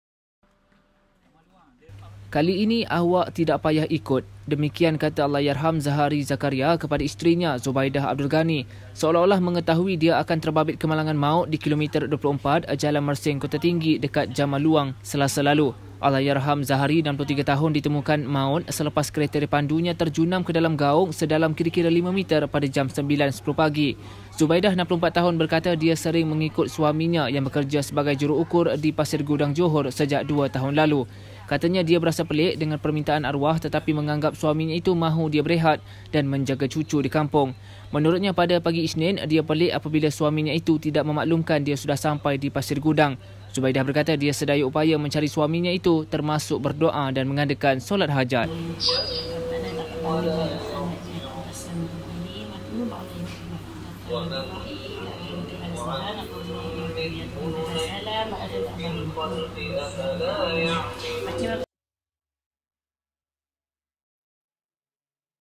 Permintaan terakhir suami
"Kali ini awak tak payah ikut." Demikian kata Allahyarham Zahari Zakaria kepada isterinya, Zubaidah Abdul Ghani seolah-olah mengetahui dia akan terbabit kemalangan maut di Kilometer 24, Jalan Mersing-Kota Tinggi, dekat Jamaluang, Selasa lalu.